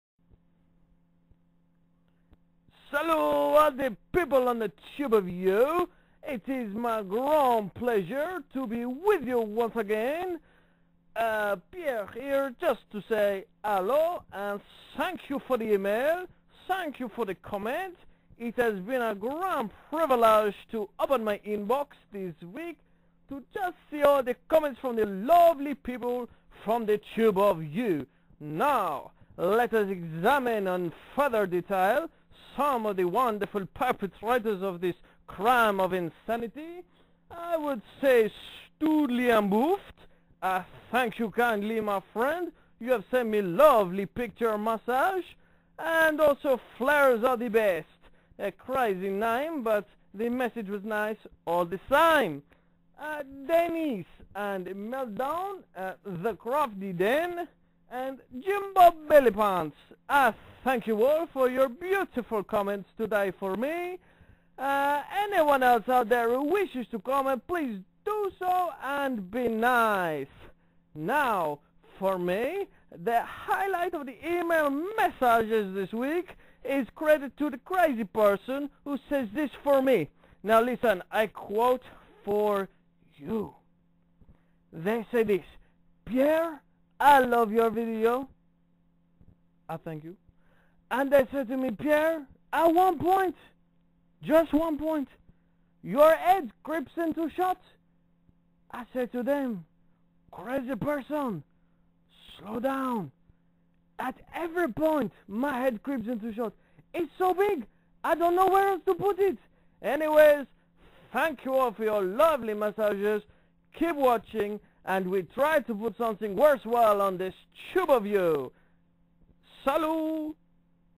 Merci for my emails!
I have had some lovely emails from you my friends... I thank you all.